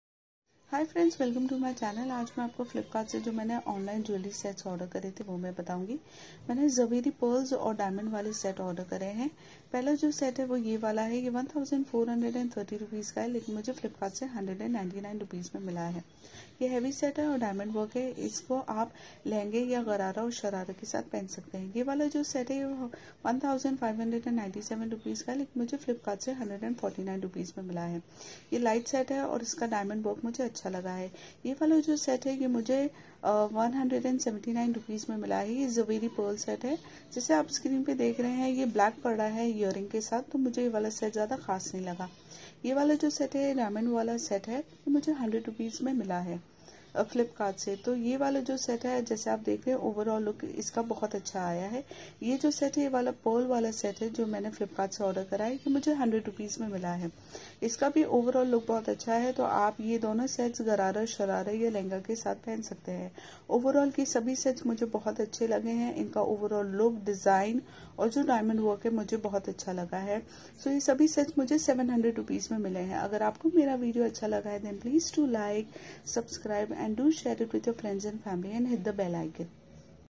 Flipkart jewellery review || online set in best price || flipkart shopping ||700₹ jewel set flipkart
i have ordered 6jewellery set from flipkart at the cost of 700 rupees. the pearl set and diamond set overall look is very nice.